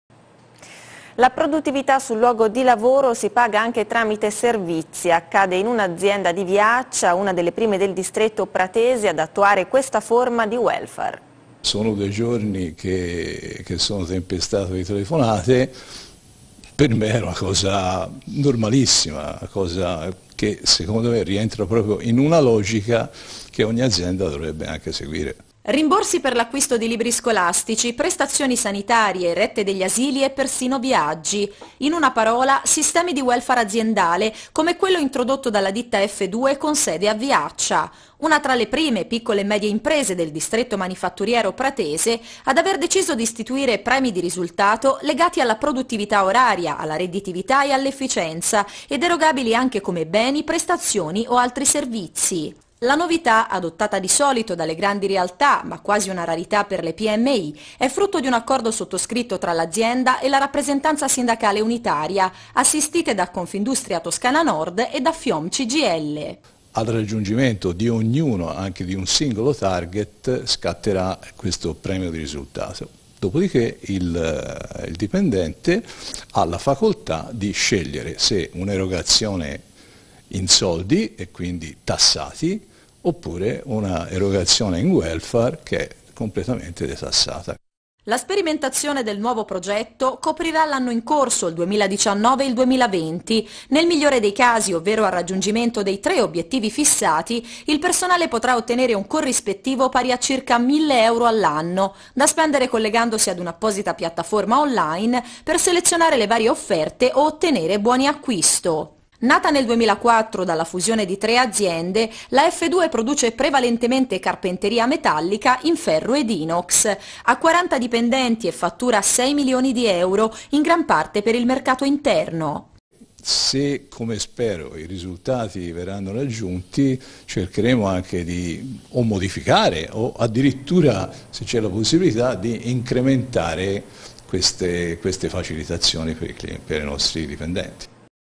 Effedue assistita da Confindustria Toscana Nord introduce il welfare
Il servizio del tg di Tv Prato sul contratto di 2° livello di Effedue srl, che, con l'assistenza di Confindustria Toscana Nord, ha introdotto il welfare aziendale. E' fra le prime PMI del distretto pratese.